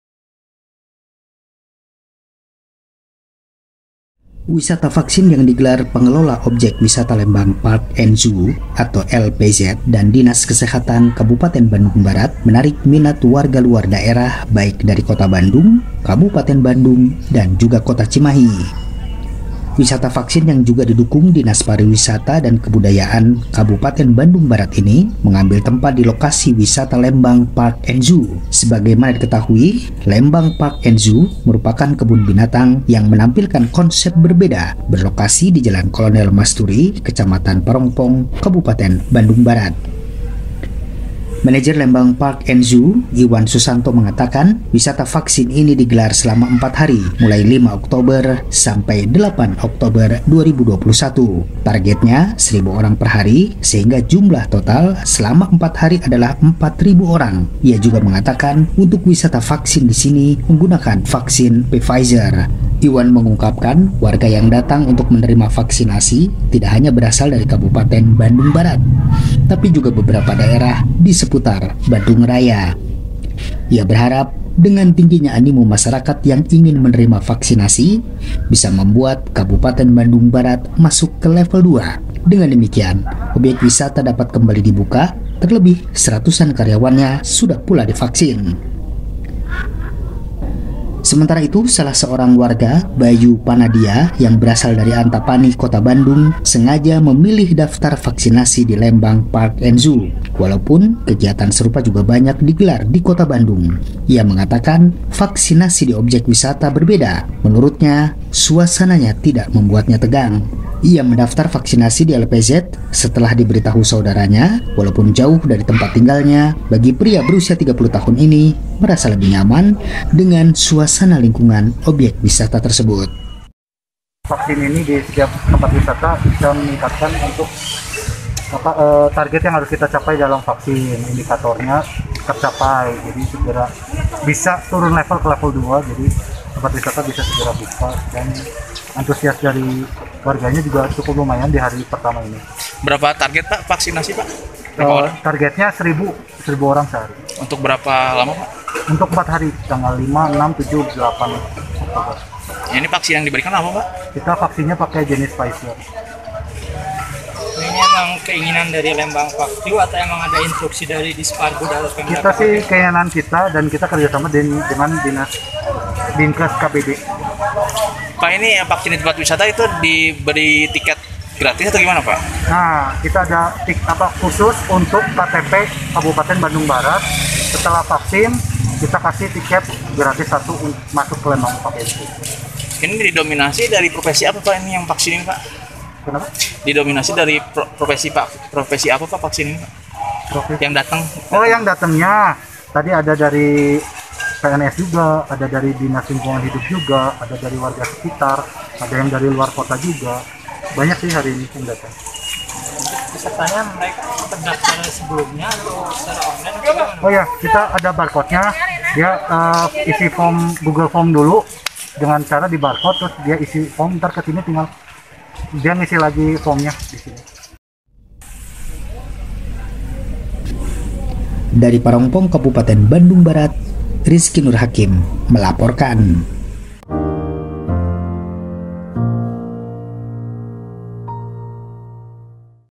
Wisata Vaksin di Objek Wisata Lembang Park and Zoo Menarik Minat Warga Luar KBB
Wisata vaksin yang digelar pengelola objek wisata Lembang Park and Zoo (LPZ) dan Dinas Kesehatan (Dinkes) Kabupaten Bandung Barat (KBB) menarik minat warga luar daerah, baik dari Kota Bandung, Kabupaten Bandung, dan Cimahi.

Wisata vaksin yang juga didukung Dinas Pariwisata dan Kebudayaan (Disparbud) KBB ini mengambil tempat di lokasi wisata LPZ.

Sebagaimana diketahui, LPZ merupakan kebun binatang yang menampilkan konsep berbeda, berlokasi di Jalan Kolonel Masruri, Kecamatan Parongpong, KBB.

Manajer Lembang Park And Zoo Iwan Susanto mengatakan wisata vaksin ini digelar selama 4 hari mulai 5 Oktober sampai 8 Oktober 2021. Tagetnya 1.000 orang per hari. Sehingga jumlah total selama 4 hari adalah, 4.000 orang. Ia juga mengatakan Untuk wisata vaksin di sini menggunakan vaksin Pfizer.

Ia mengungkapkan, warga yang datang untuk menerima vaksinasi tidak hanya berasal dari KBB, tapi juga beberapa daerah di seputar Bandung Raya.

Ia berharap dengan tingginya animo masyarakat yang ingin menerima vaksinasi bisa membuat KBB masuk level 2. Dengan demikian objek wisata dapat kembali dibuka, terlebih seratusan karyawannya sudah pula divaksin.

Salah seorang warga, Bayu Panadia yang berasal dari Antapani, Kota Bandung sengaja memilih daftar vaksinasi di LPZ walaupun kegiatan serupa juga banyak digelar di Kota Bandung. Ia mengatakan vaksinasi di objek wisata berbeda. Menurutnya suasananya tidak membuatnya tegang.

Ia mendaftar vaksinasi di LPZ setelah diberitahu saudaranya. Walaupun jauh dari tempat tinggalnya, bagi pria berusia 30 tahun ini merasa lebih nyaman dengan suasana lingkungan objek wisata tersebut.***